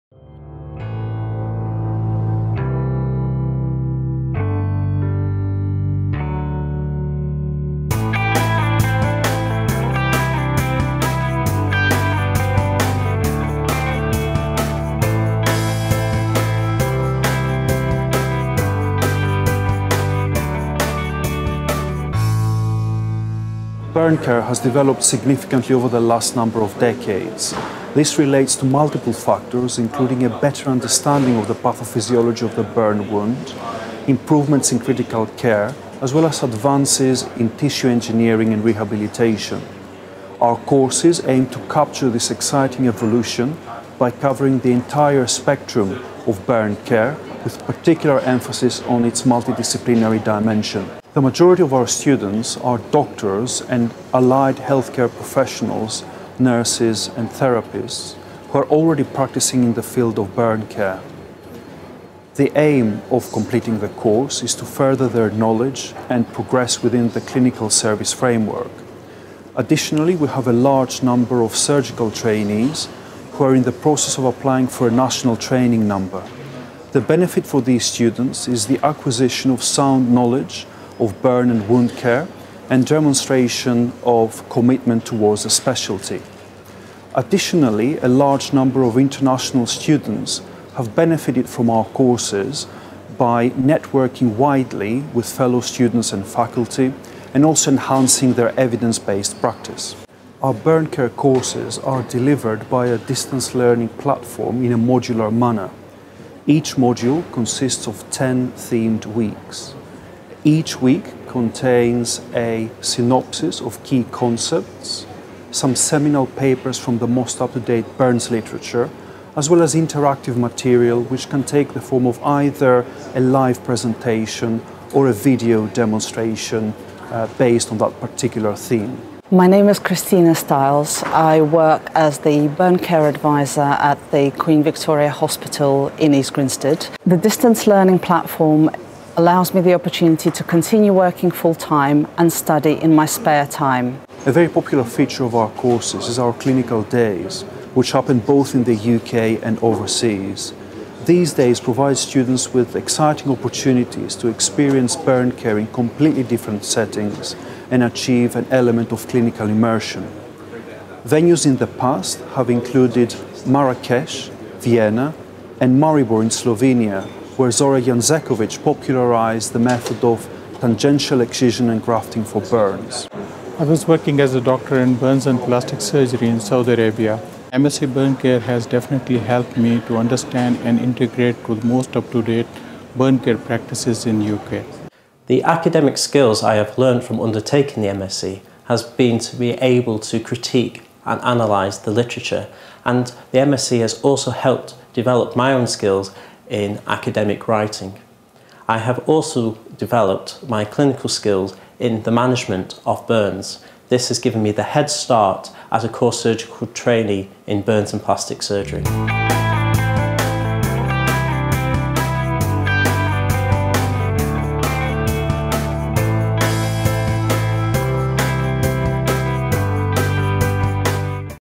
MSc / PGCert Burn Care – Distance Learning
Find out about studying MSc Burn Care - distance learning at QMUL. The video includes course content information, career pathways and highlights all the benefits of studying this exciting subject area.

[Add playlist link if necessary]

In this video [insert description]